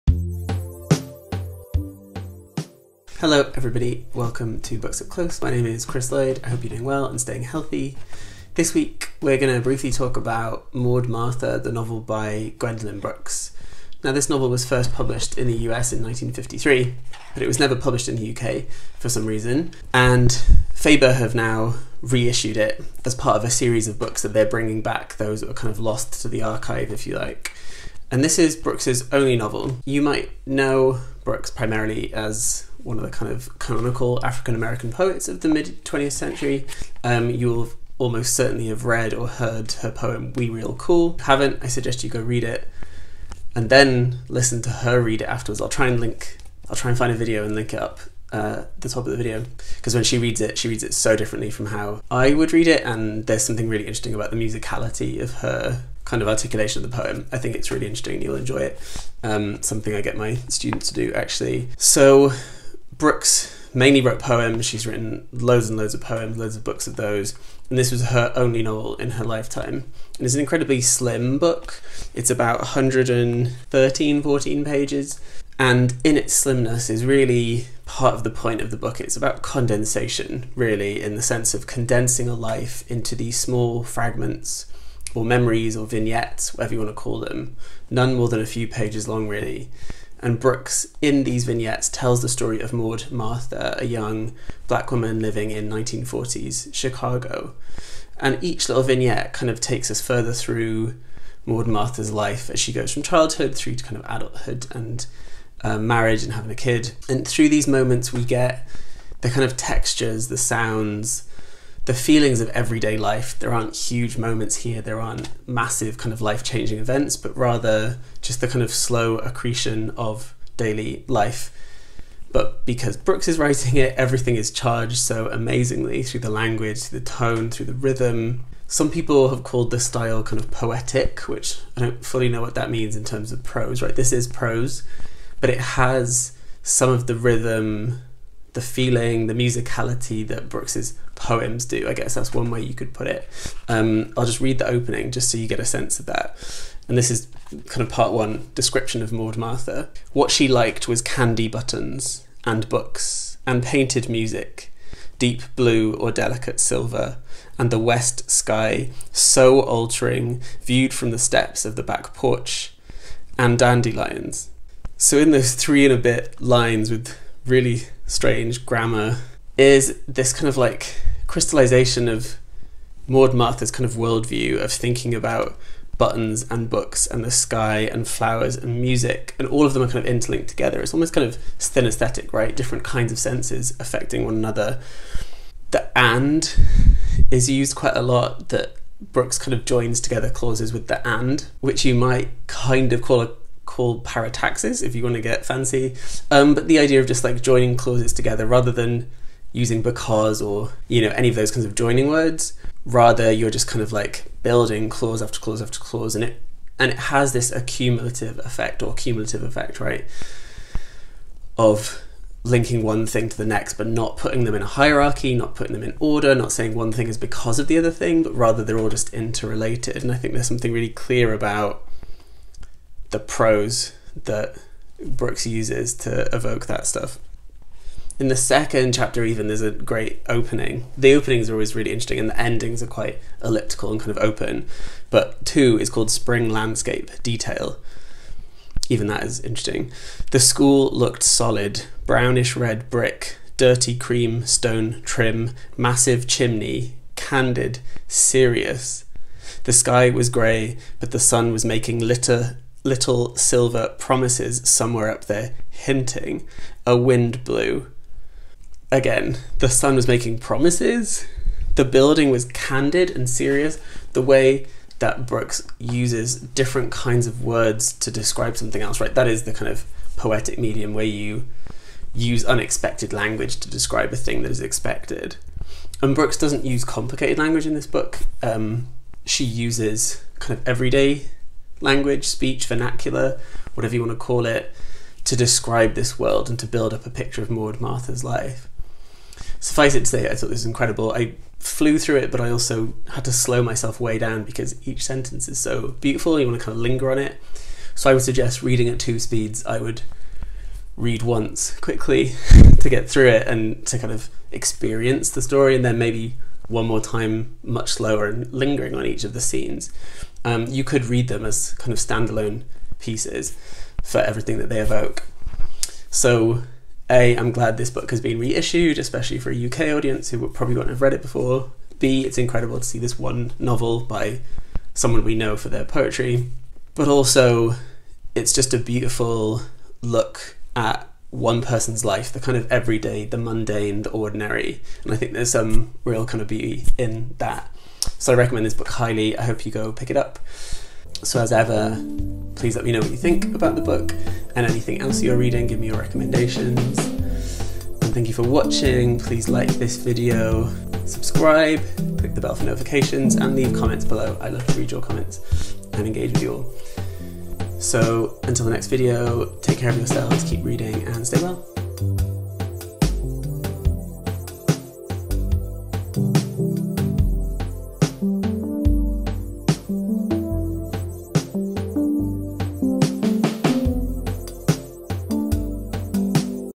Ep 32 | BOOKS UP CLOSE | Gwendolyn Brooks, Maud Martha
Episode 32 of BOOKS UP CLOSE is here!

Dr Chris Lloyd reviews Gwendolyn Brooks' only novel, Maud Martha (1953), re-issued in 2022 by Faber.

Tell me what you think of this book! You can see what else I'm reading